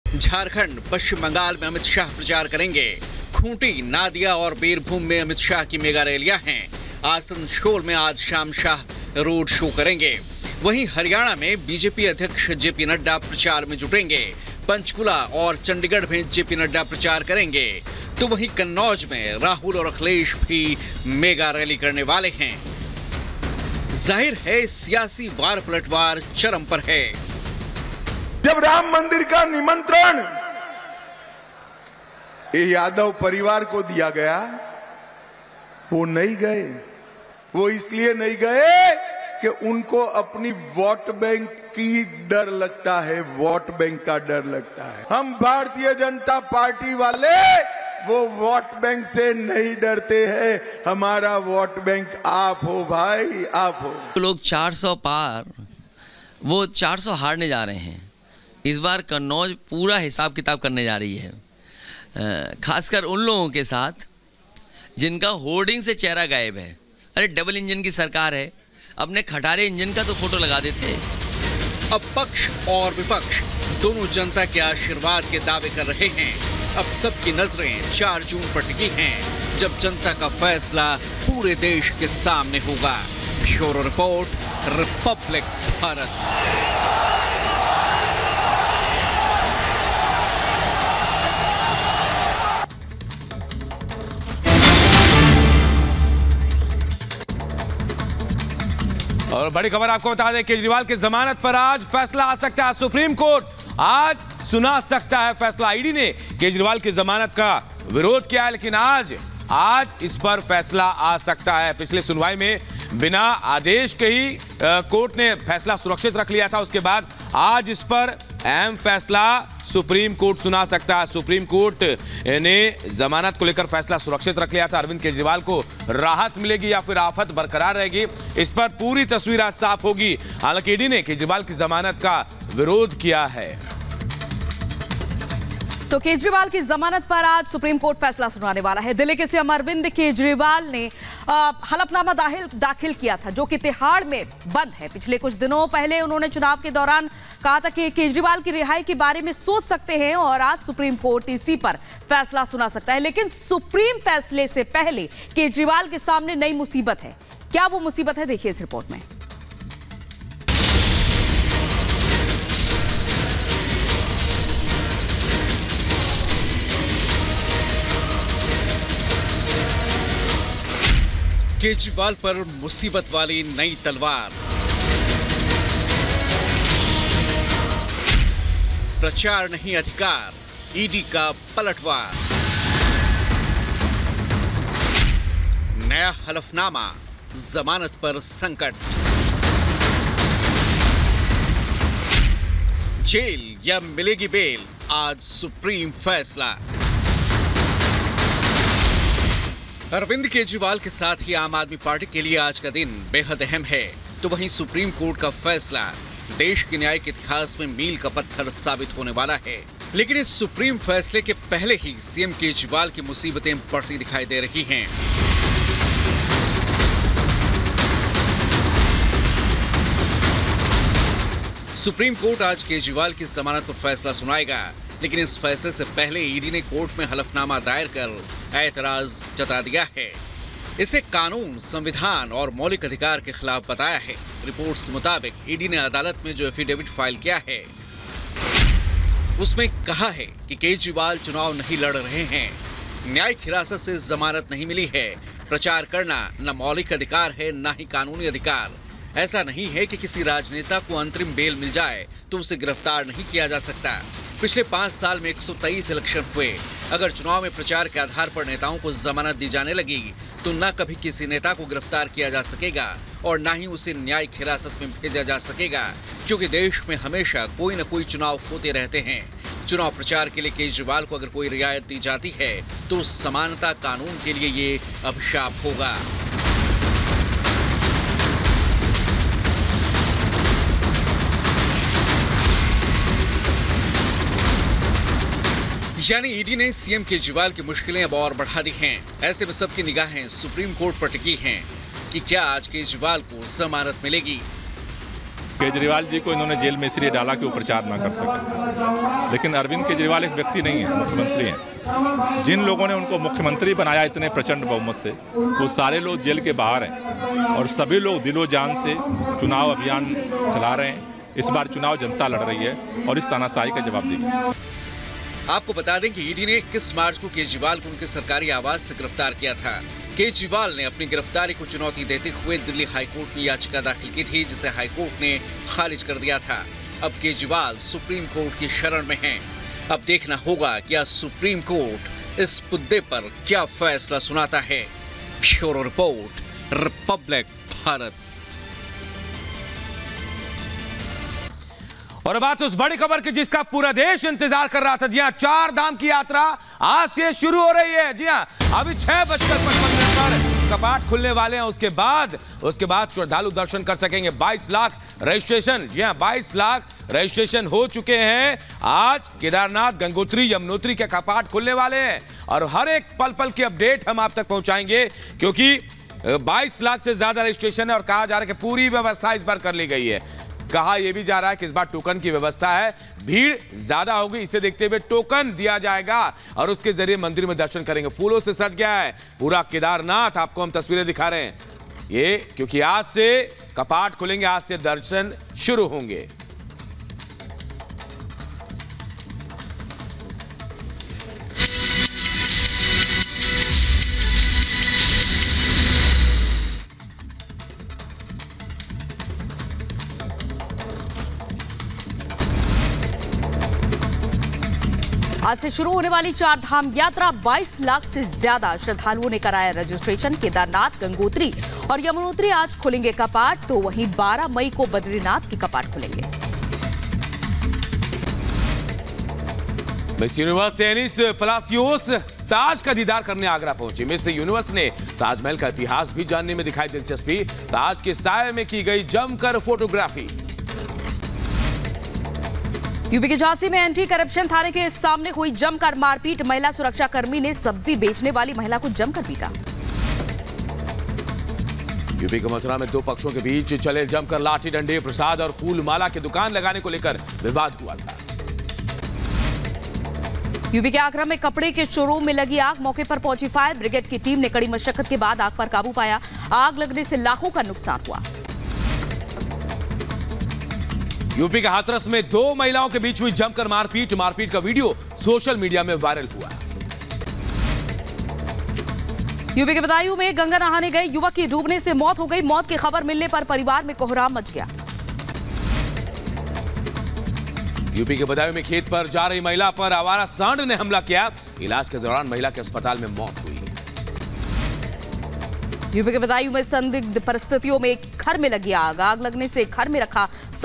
Lok Sabha Election 2024: जानिए आज कौन से दिग्गज कहां करेंगे प्रचार | R Bharat
लोकसभा चुनाव 2024 के चौथे चरण के लिए प्रचार अभियान तेज है. देश भर में सोमवार यानी 13 मई को 10 राज्यों और केंद्र शासित प्रदेशों में फैले 96 लोकसभा निर्वाचन क्षेत्र में मतदान होने जा रहा है. इसे लेकर तमाम सियासी पार्टियां धुआंधार रैली और प्रचार अभियान कर रही है. प्रधानमंत्री नरेंद्र मोदी जेपी नड्डा, राहुल गांधी, प्रियंका गांधी और अन्य राजनेताओं द्वारा देश भर में आज रैली किया जाने वाला है.